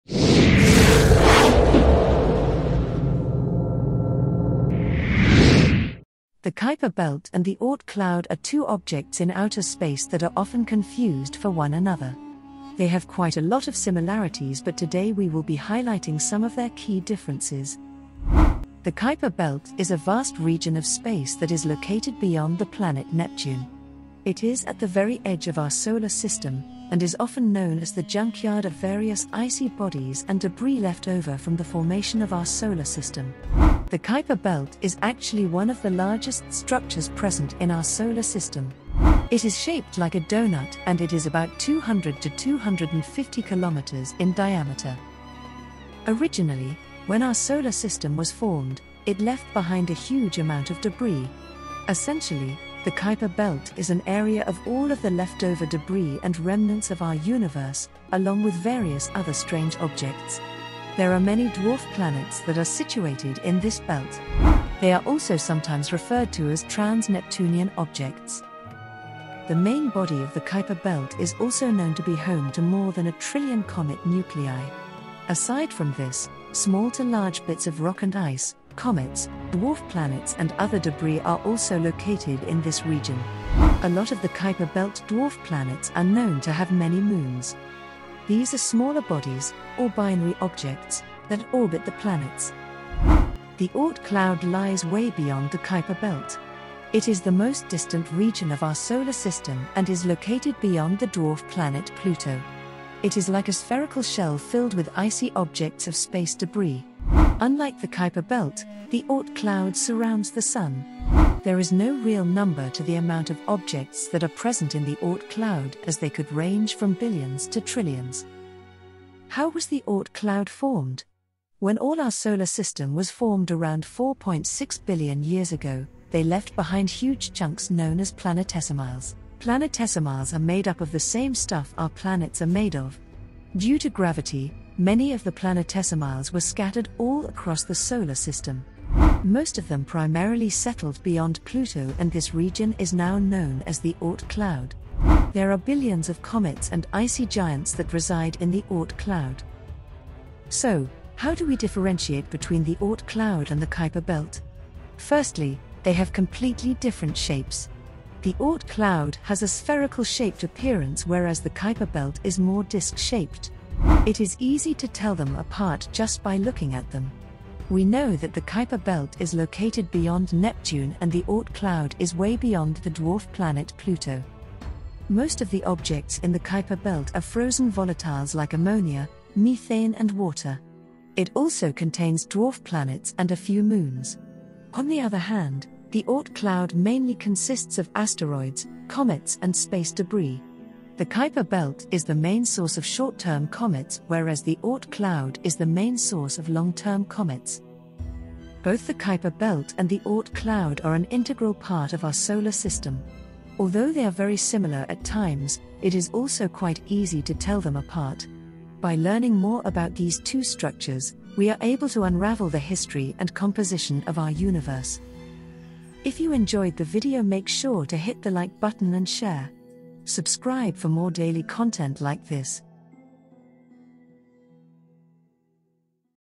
Kuiper Belt And Oort Cloud Explained
The Kuiper Belt and the Oort Cloud are two structures in outer space that are quite alike. They are both formed from the remnants of our early universe and they both contain a lot of objects such as comets and asteroids. What are some of their key differences?